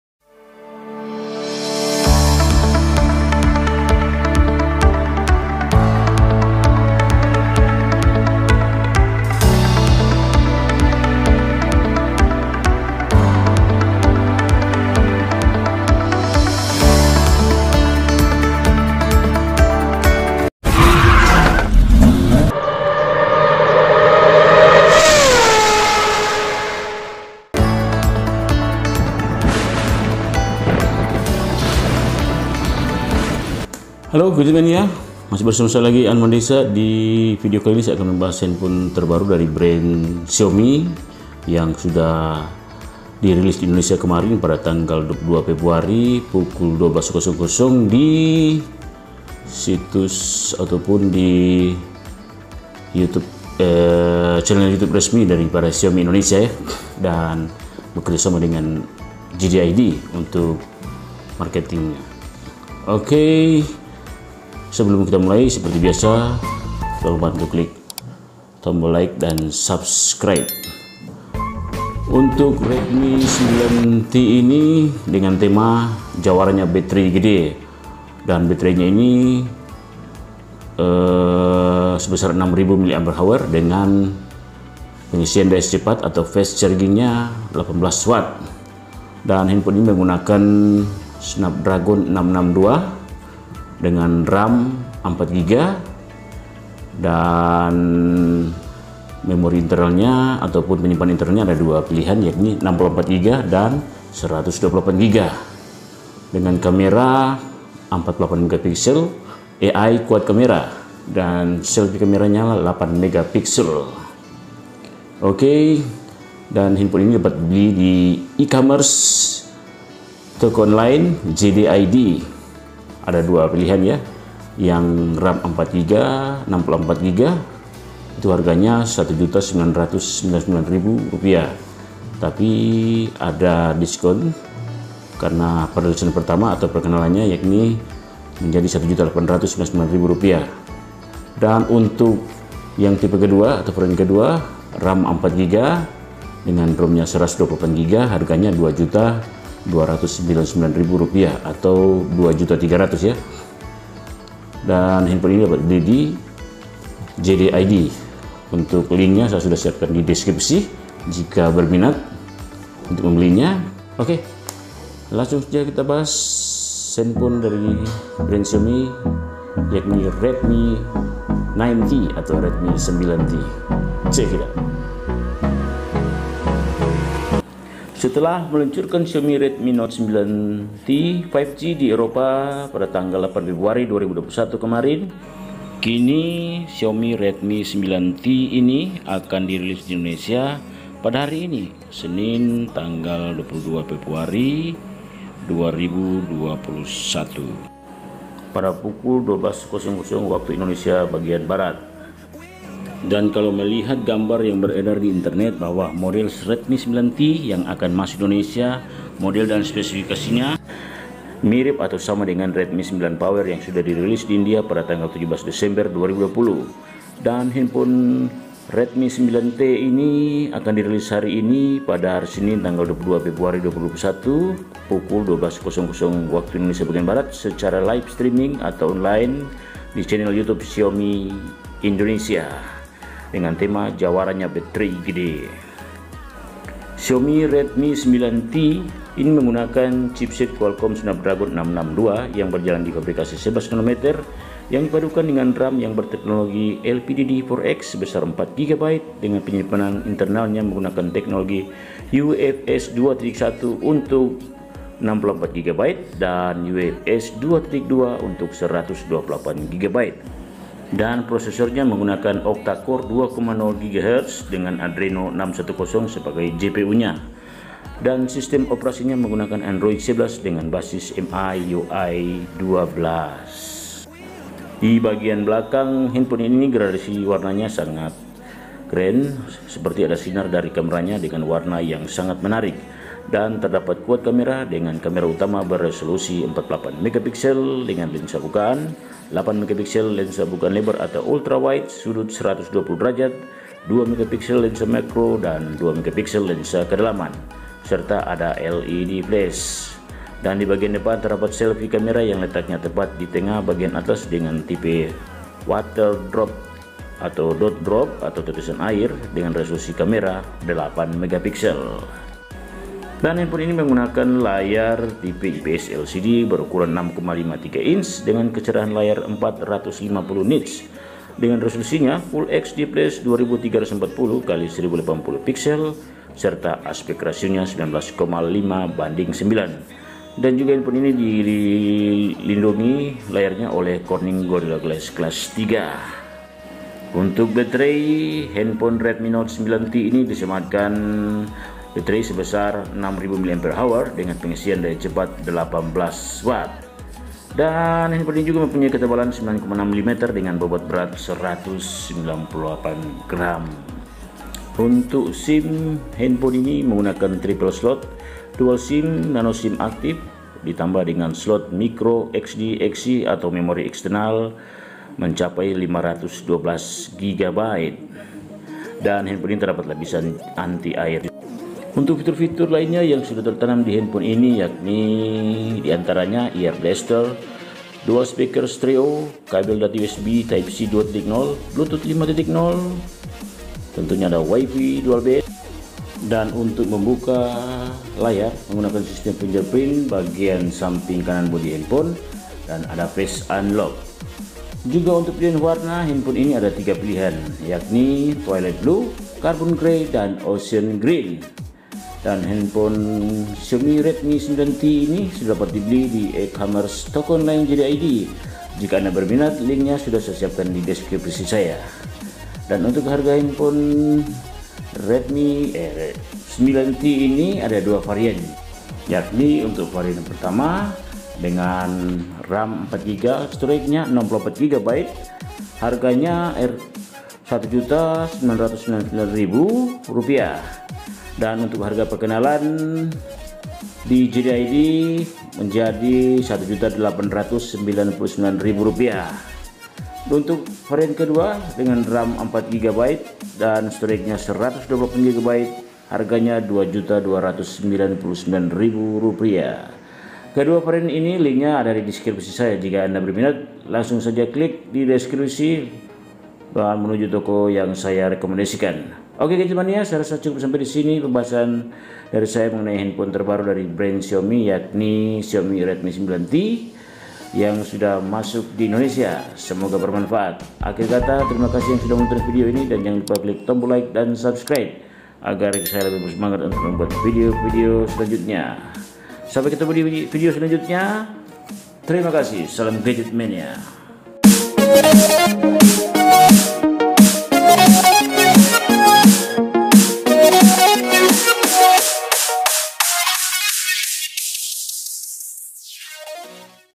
Xiaomi Redmi 9T | HP 2 jutaan RAM 4GB/64/128GB Design Keren Review
Xiaomi Redmi 9T
Untuk pembelian handphone ini hanya di e-commerce JD.id Toko Online linknya di bawah ini langsung ke Tokonya ya :

Jd.id Xiaomi Redmi 9T RAM 4/64GB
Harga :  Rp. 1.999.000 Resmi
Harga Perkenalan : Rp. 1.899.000

JD.id Xiaomi Redmi 9T RAM 4/128GB :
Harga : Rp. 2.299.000

Spesifikasi lengkap Xiaomi Redmi 9T
Prosesor / Performance
Chipset : Qualcomm Snapdragon 662 (11nm)
RAM LPDD4X 4GB
UFS 2.1 for 64GB
UFS 2.2 for 128GB
OS : Android 11
UI : MIUI 12.

Rear Camera / Kamera Belakang :
AI Quad Camera
48MP Main Camera (Wide Lens)
8MP Ultrawide Lens 120°
2MP Macro lens
2MP Depth lens
LED flash

Kamera Depan :
8MP Selfie Camera

Display / Layar :
IPS LCD 6,53 inch dengan klayar
450 nits screen brightness
Resolution : FHD+ 2340 x 1080 pixels, aspec ratio 19,5 : 9
Protection : Gorilla glass class 3.
Baterai Besar / Big Baterai :
6000mAh baterai
18Watt Fast charging

Colors / Warna 3 Varian :
Twilight Blue
Carbon Grey
Ocean Green

Price / Harga Redmi 9T ada 2 Varian :
RAM 4GB/64GB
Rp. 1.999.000
Harga Perkenalan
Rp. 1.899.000

RAM 4GB/128GB :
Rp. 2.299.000

Senjata alias perlengkapan perang :
- Vivo V20
- Aplikasi Kinmaster
- Tripod